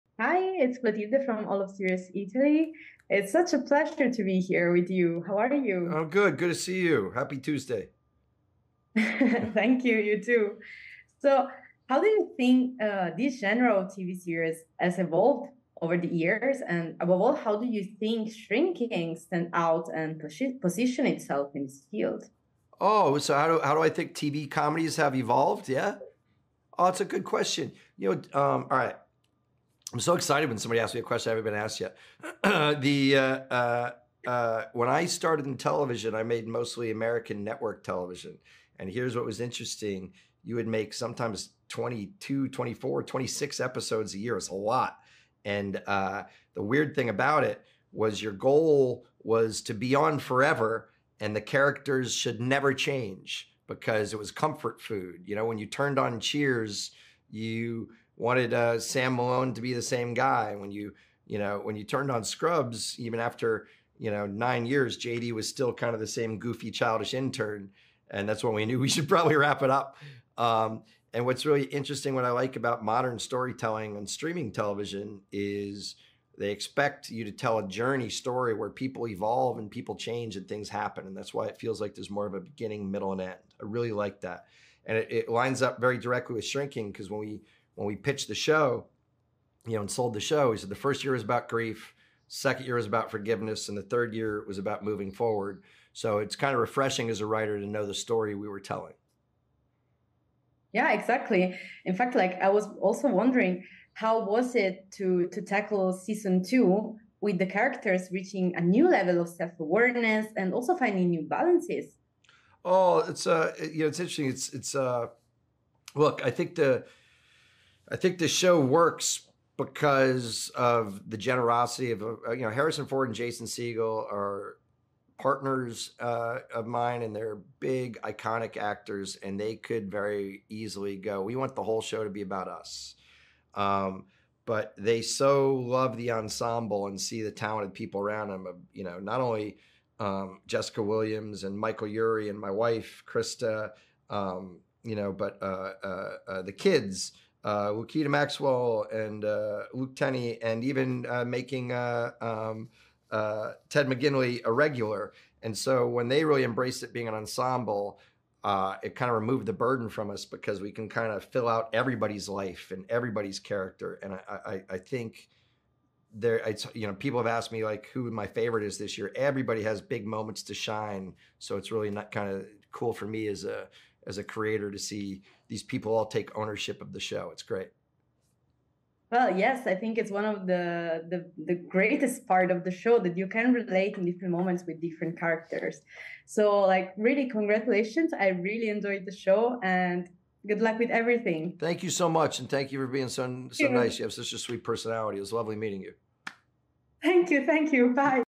Shrinking 2 - Intervista a Bill Lawrence
La nostra intervista a Bill Lawrence, già creatore di Scrubs e produttore esecutivo di Ted Lasso, in occasione della seconda stagione di Shrinking (di cui è produttore esecutivo) in arrivo il 16 Ottobre su Apple TV+